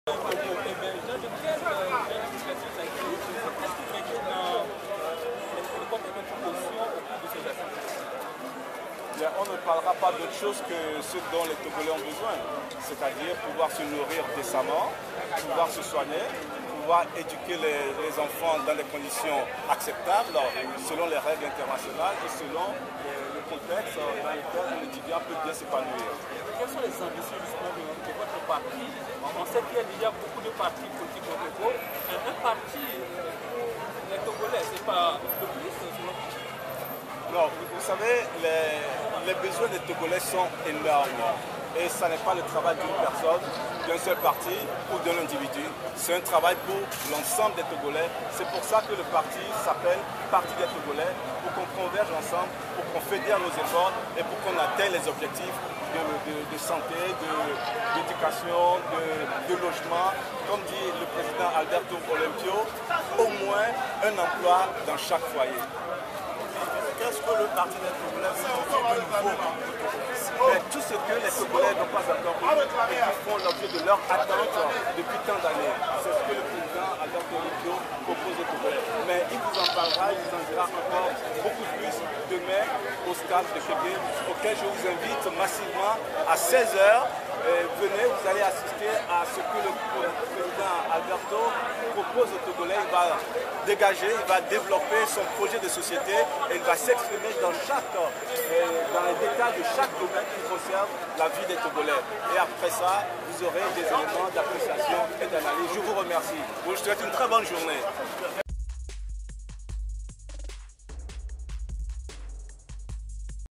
Nathanaël Olympio: on ne parlera pas d'autre chose que ce dont les Togolais ont besoin
1/8/2014 - Le Parti des Togolais d’Alberto Olympio, 104e et dernier né des partis politiques du Togo,  tient son premier congrès au Grand Rex de Lomé. Nathanaël Olympio, un des responsables du parti explique les objectifs de ce premier congrès.